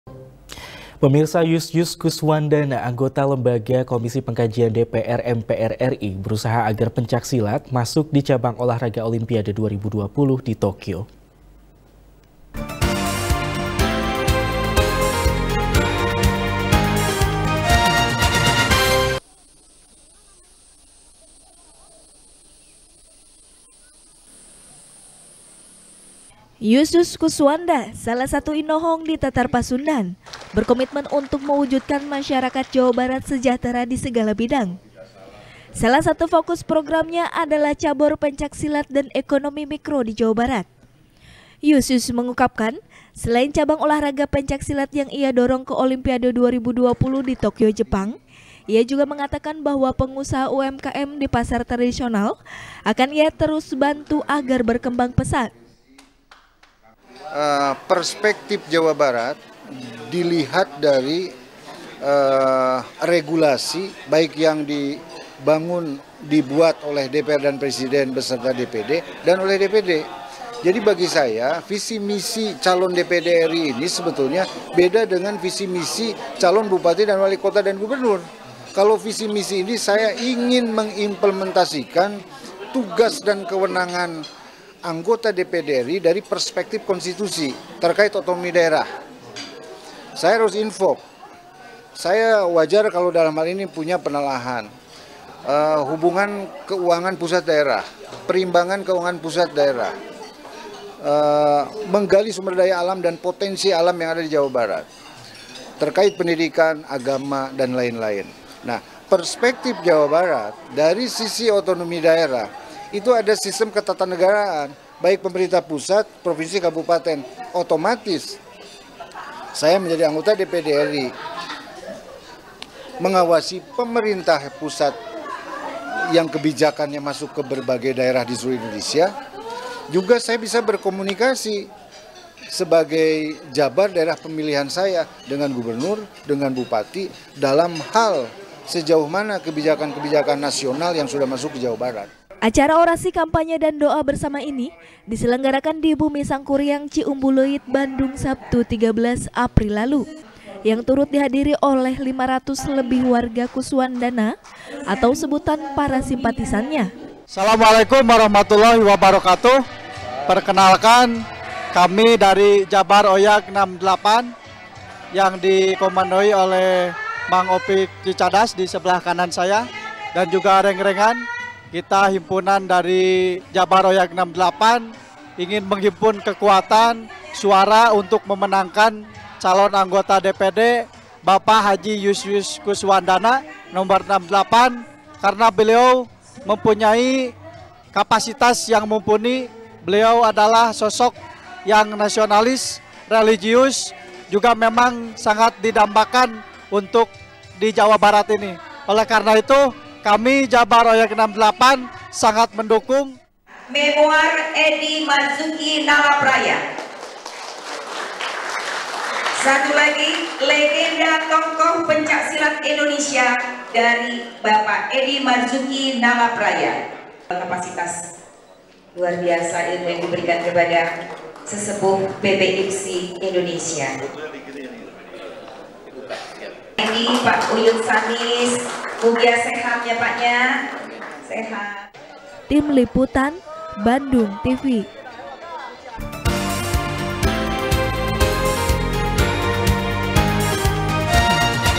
Yusyus Dorong Pencak Silat Ke Olimpiade 2020 | SBR | BANDUNGTV
SBR | BANDUNGTV | IRFAN RAMANDA | Yusyus Kuswandana salah satu inohong di Tatar Pasundan, berkomitmen untuk mewujudkan masyarakat Jawa Barat sejahtera di segala bidang. Salah satu fokus programnya adalah cabor pencak silat dan ekonomi mikro di Jawa Barat. Yusyus mengungkapkan, selain cabang olahraga pencak silat yang ia dorong ke Olimpiade 2020 di Tokyo Jepang. Ia juga mengatakan bahwa pengusaha UMKM di pasar tradisional akan terus ia bantu agar berkembang pesat.